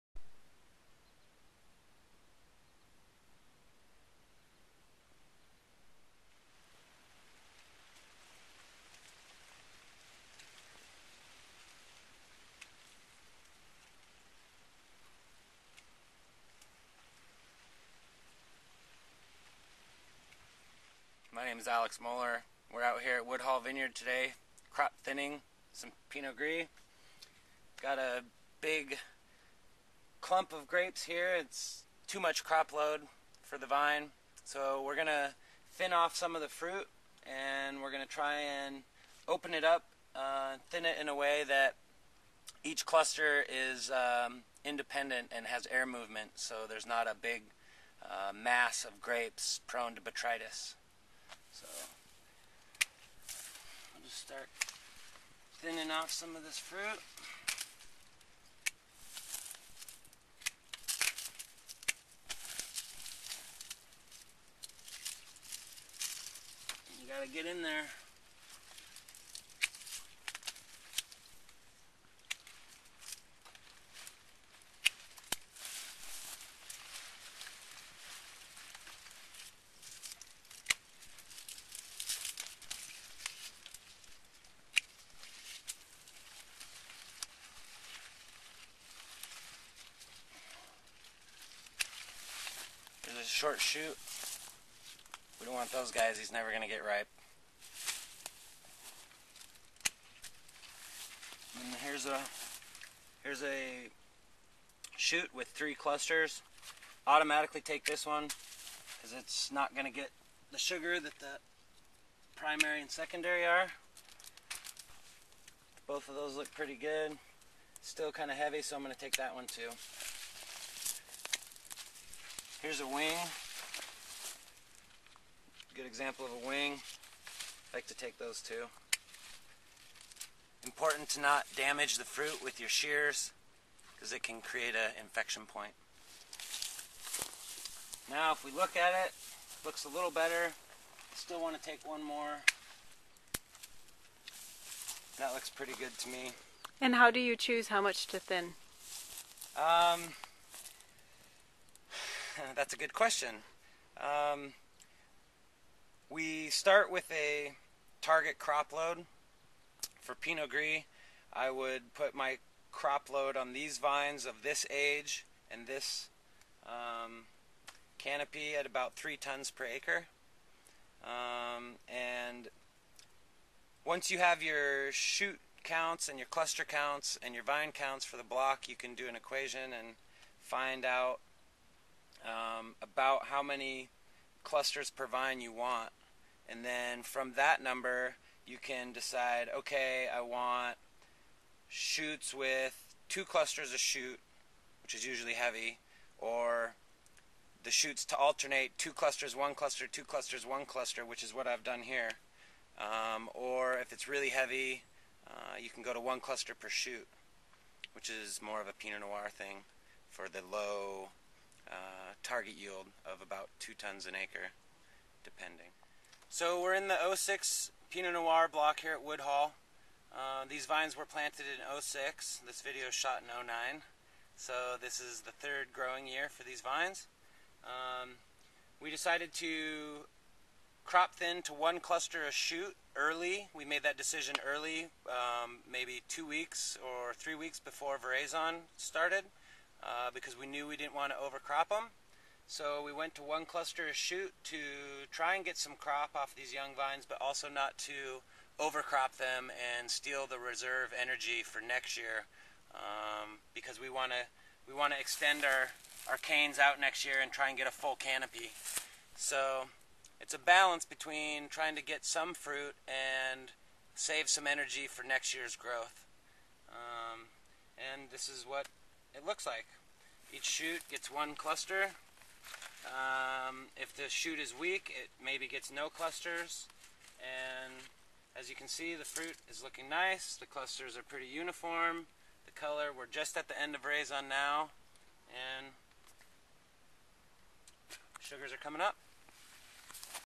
Crop thinning in cool climate vineyards
Crop reduction is a common practice in cool climate viticulture where seasonal weather restricts heavy crop levels. This video summarizes the procedures for crop reduction through cluster thinning at Oregon State University's teaching and research vineyard, Woodhall.